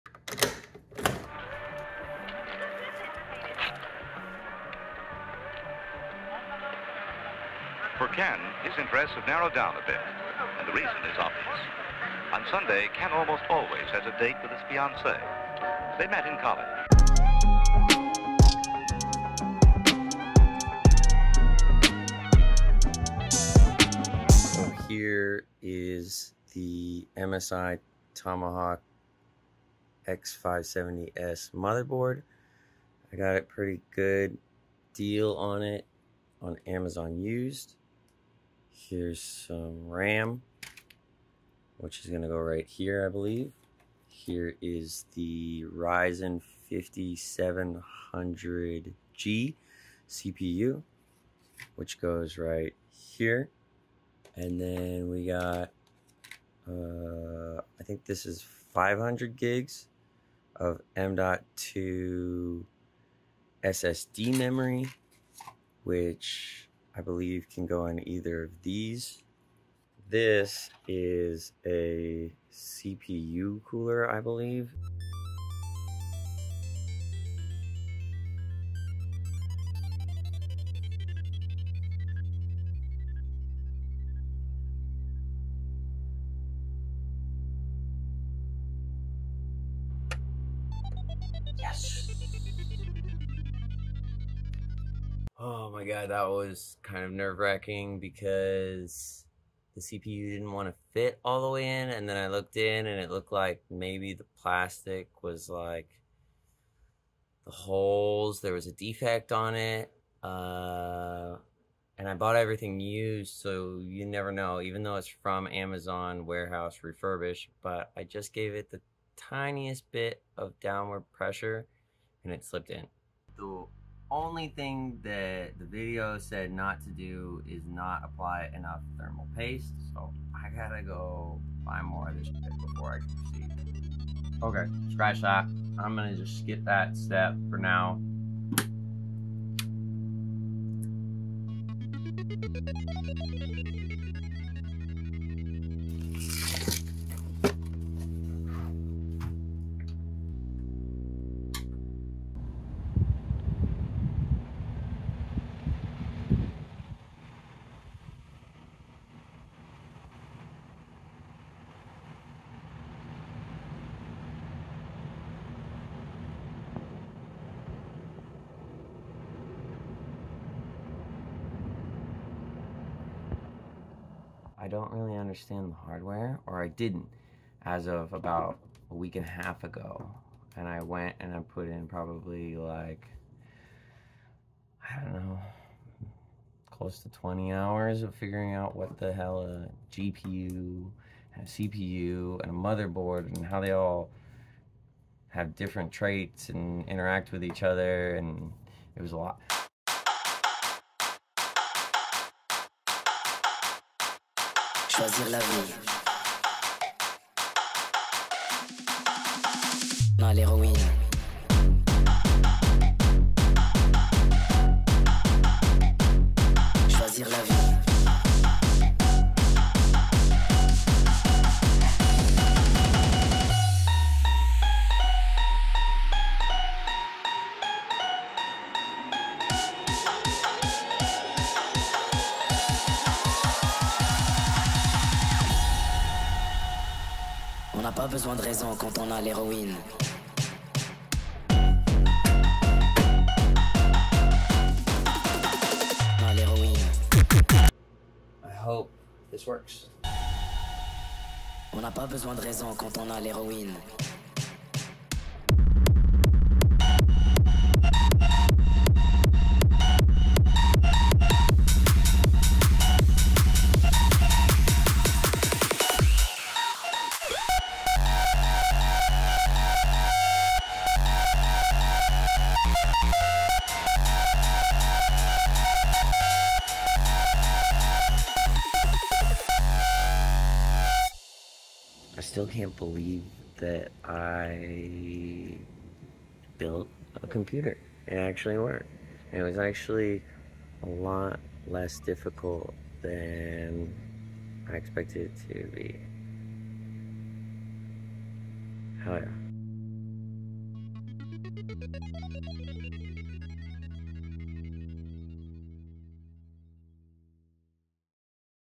DL92 Editing PC Build 2
Learning how to build an editing PC using an AMD Ryzen 5700g chip & a MSI Tomahawk X570S motherboard